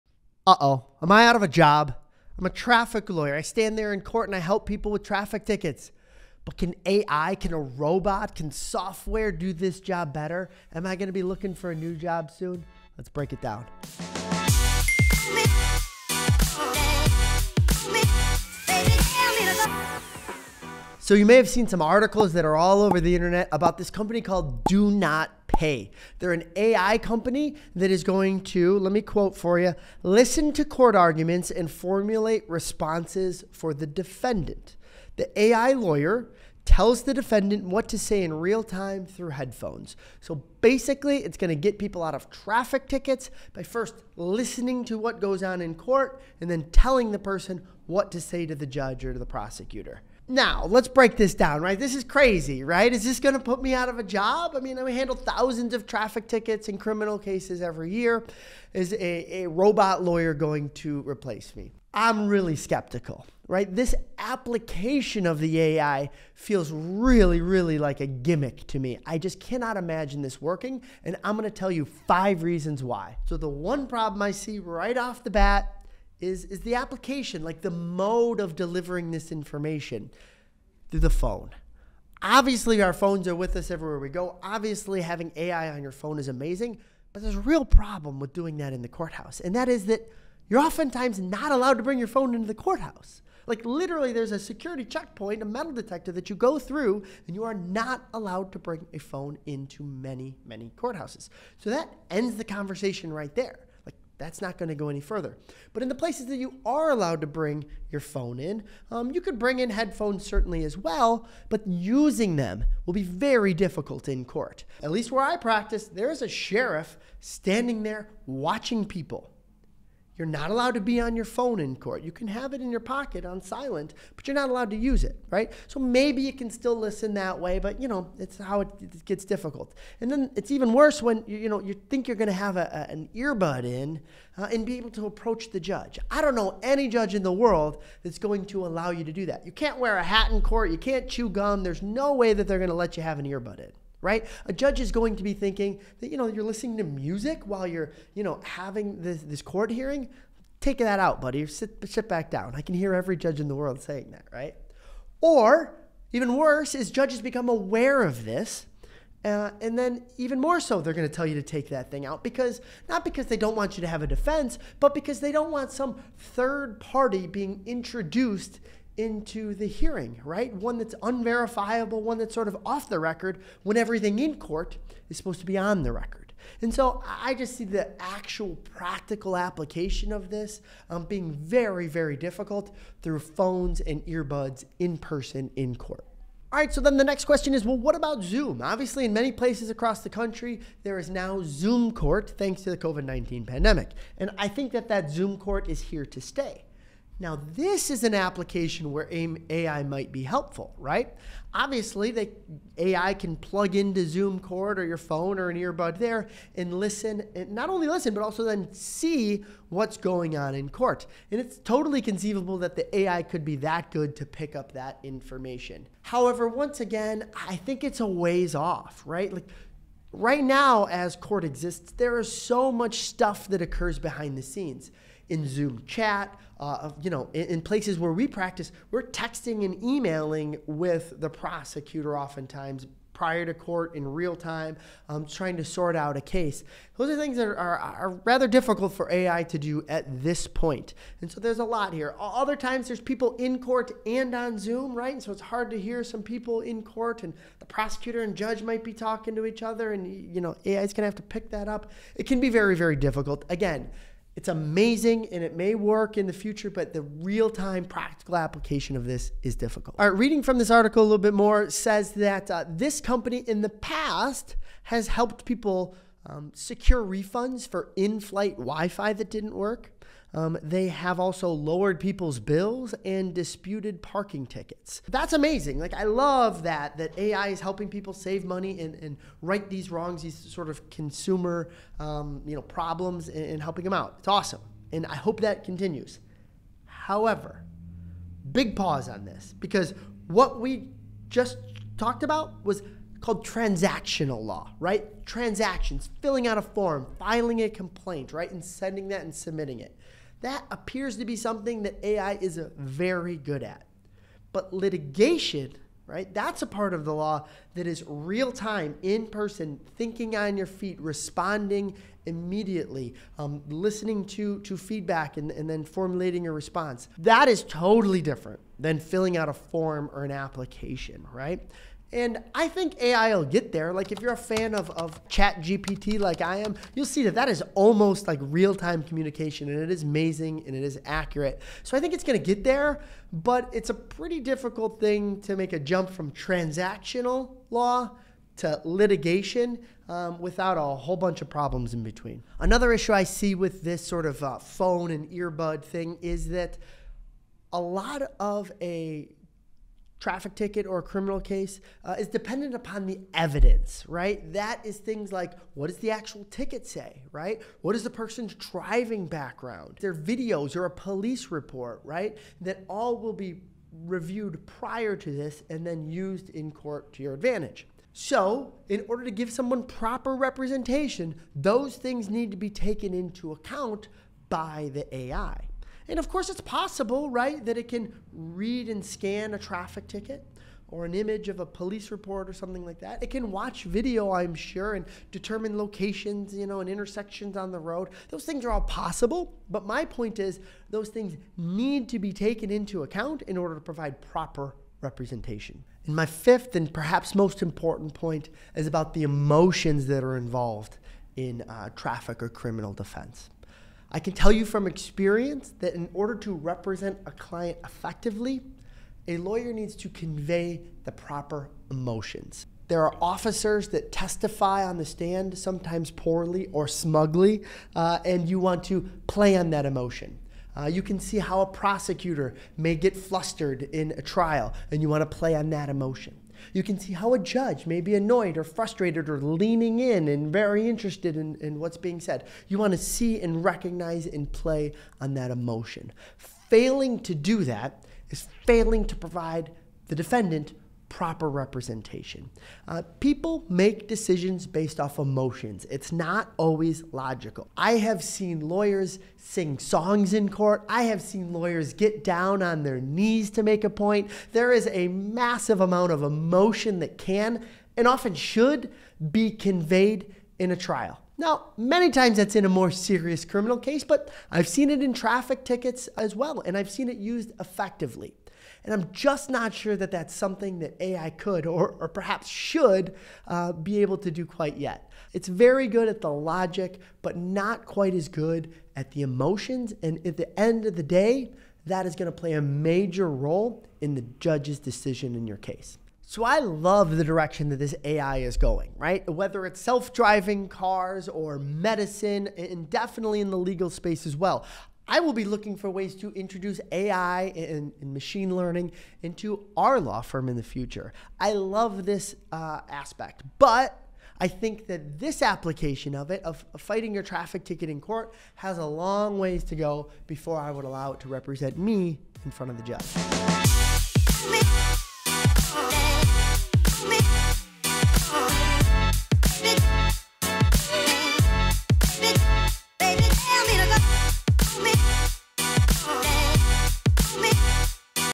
Is this going to make traffic LAWYERS OBSOLETE?
Articles have been popping up all over the internet about DoNotPay's artificial intelligence robot lawyer. As a DUI and Traffic law firm we see firsthand what this AI lawyer is going to have to do and foresee some drawbacks to its implementation in the real world. Although we're skeptical, we are excited to see how this plays out!

Chapters:

00:00 Intro
01:16 Mode of Delivering Information
03:06 What about Zoom?
04:50 Transactional Law vs Litigation
05:46 Dependent on Evidence
06:35 Emotion
08:05 Conclusion